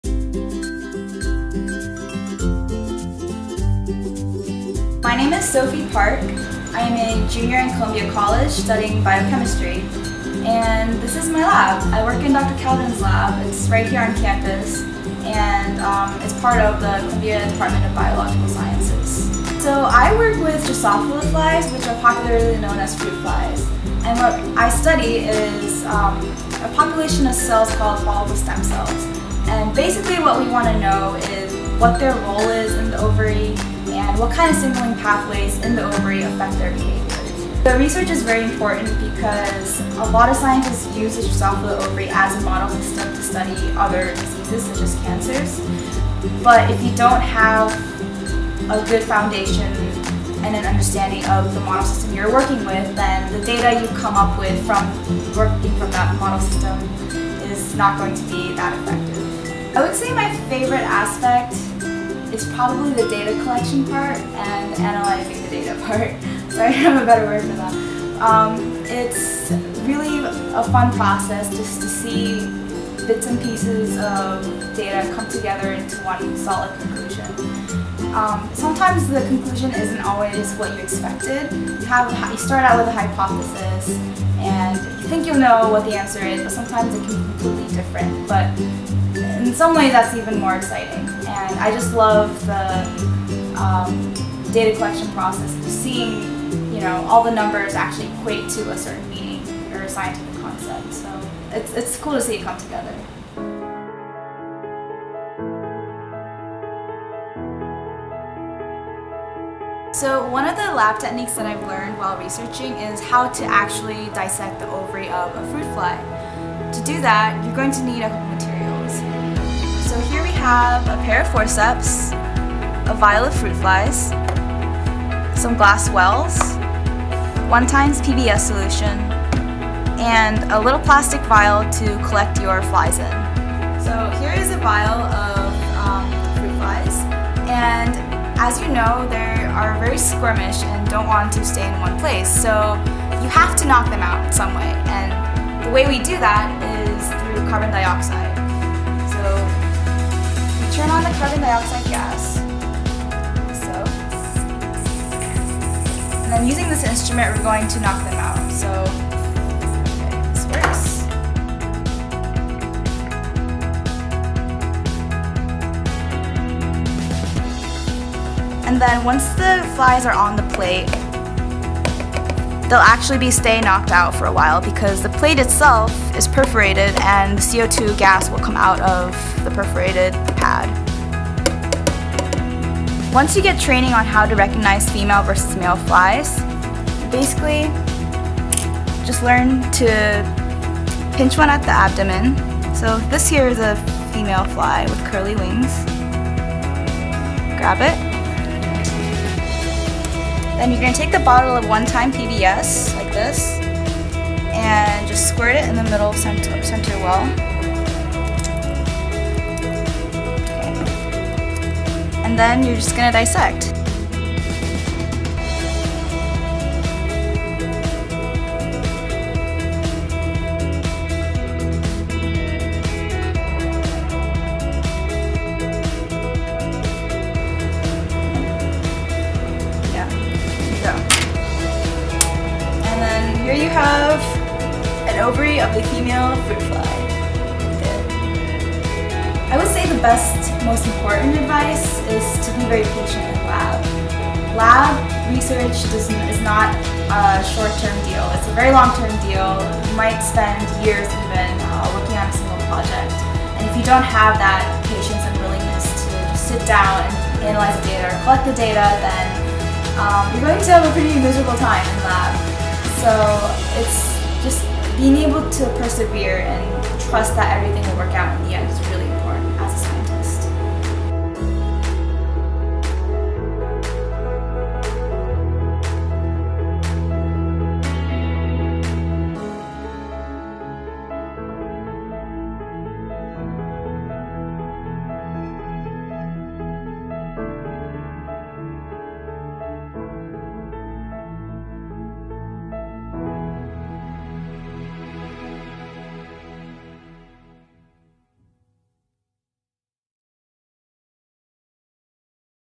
Sophie Park CC'16 | Scientists in the Making
We are beyond excited to launch the first installment of Columbia Science Review's Spread Science video series! Our first interviewee was Sophie Park, CC '16, who talks about her research and experience with fruit fly ovaries at Columbia University's Department of Biological Sciences.

Video crew: Sophie Park, Amy Xia, Audrey Shi